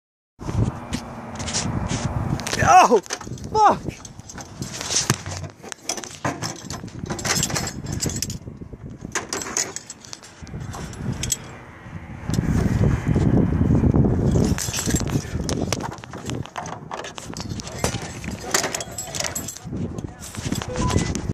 Hawk attacking guy on cell tower 2
Hawk attacks me on a cell tower.